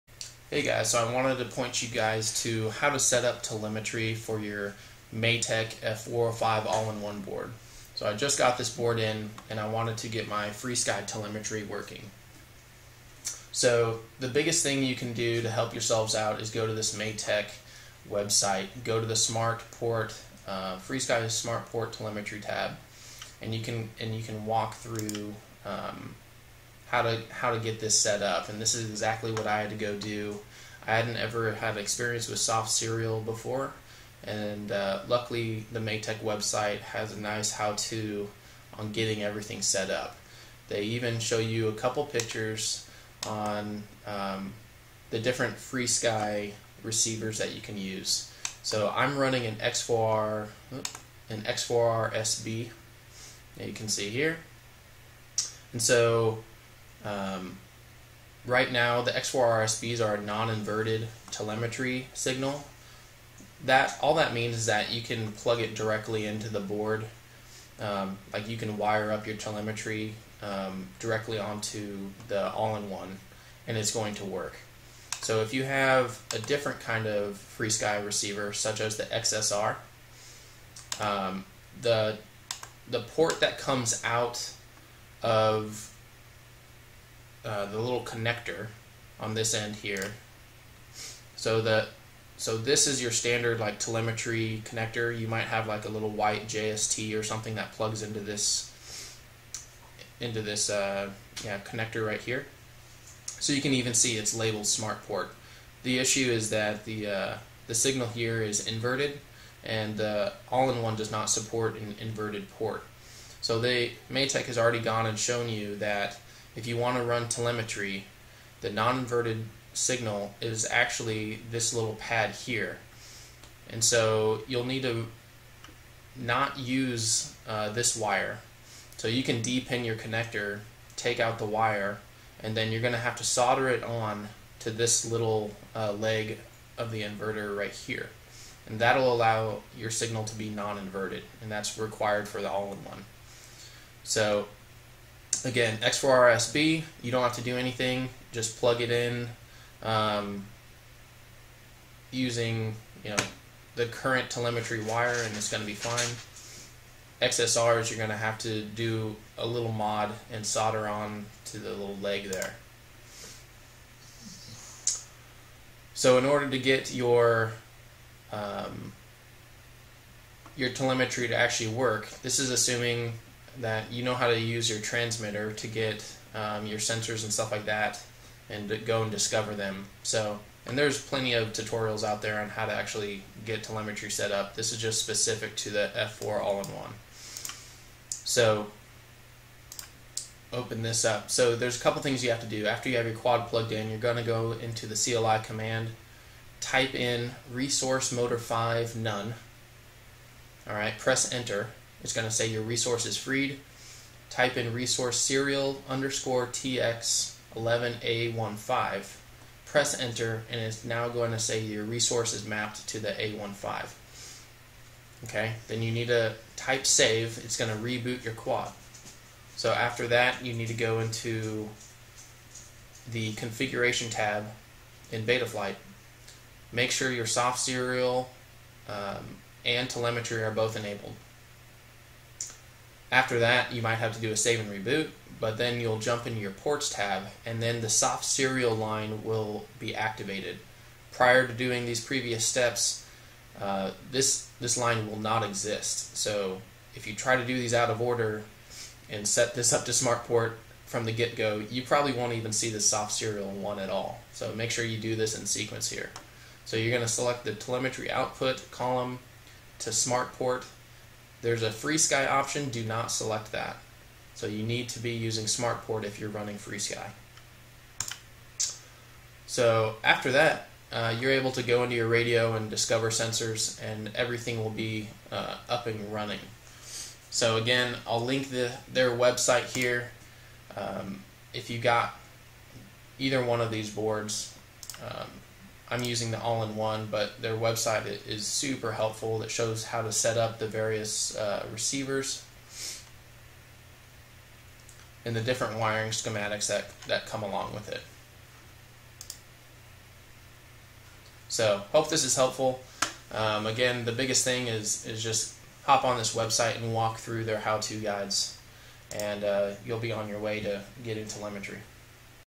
How To: Matek F405 AIO + FrSky Telemetry
Setup guide for the Matek F405 AIO flight controller and how to enable FrSky telemetry.

Matek's website has a great "walkthru" which I reference throughout the video -- hope you find this useful!